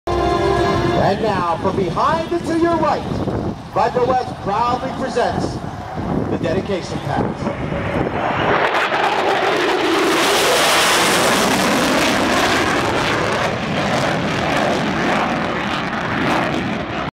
General Dynamics F-16C Fighting Falcon Performs A Dedication Pass At KHIO
A General Dynamics F-16C Fighting Falcon performs a dedication pass at the Hillsboro Airport (KHIO) during the 2011 Oregon International Airport (KHIO).